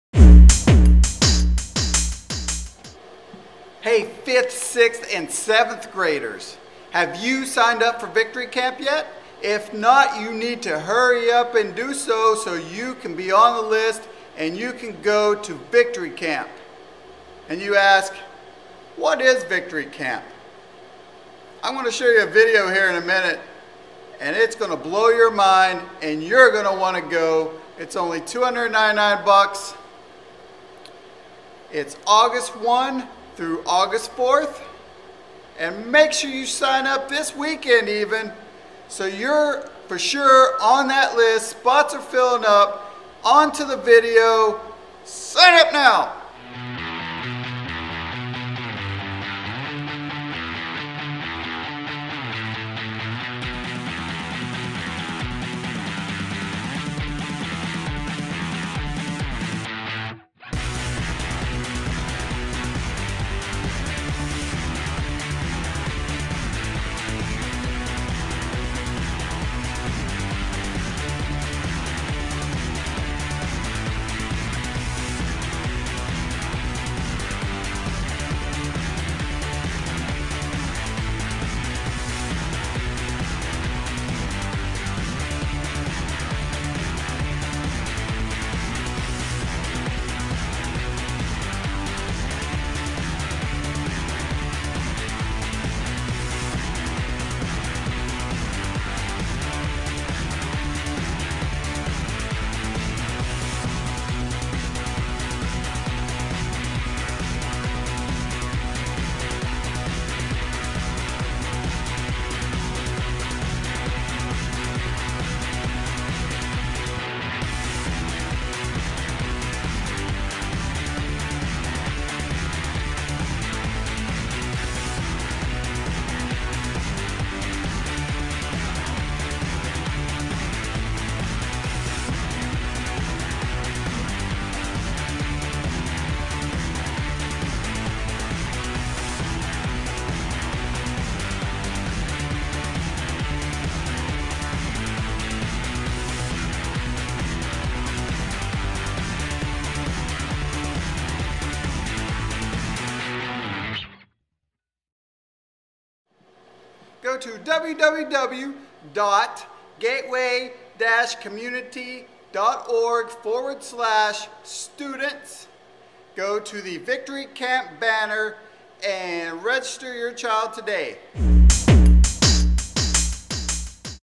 Victory Camp Info
Victory Camp is located in Alvin, TX and is the chosen destination for this years Junior High Camp! and click on the Victory Camp banner to register your student!